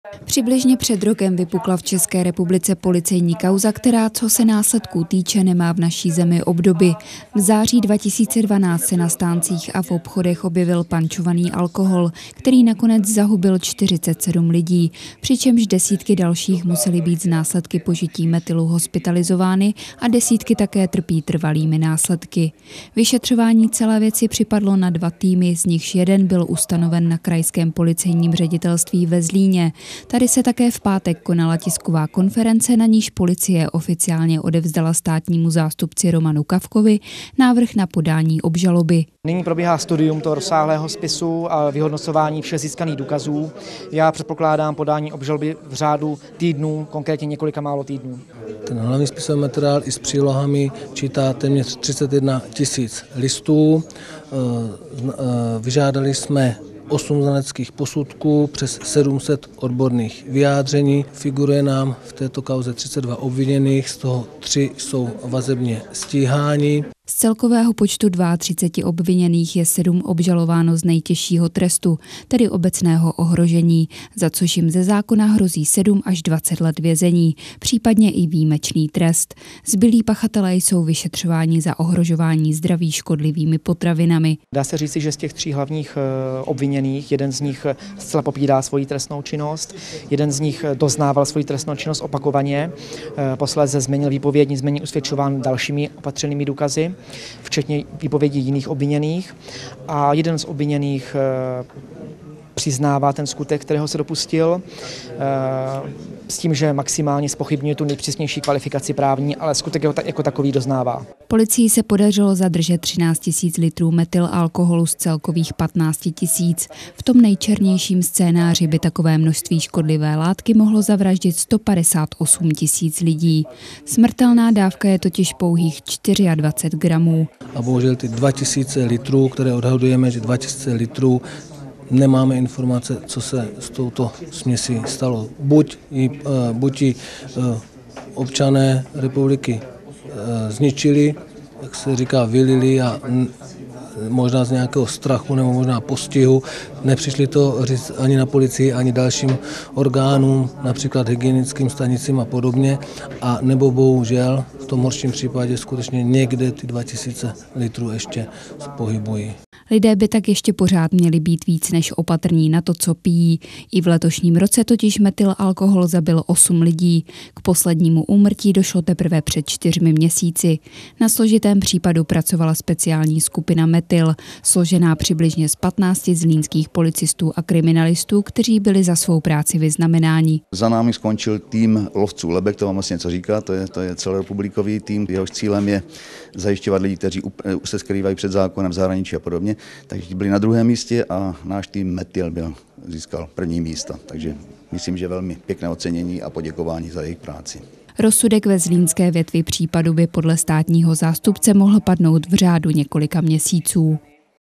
TV Slovácko: Zlín - Oficiální obžaloba v kauze metyl
Kauza metyl se po více jak roce konečně dostane k soudu. Krajské policejní ředitelství ve Zlíně totiž slavnostně předalo státnímu zástupci návrh na podání obžaloby.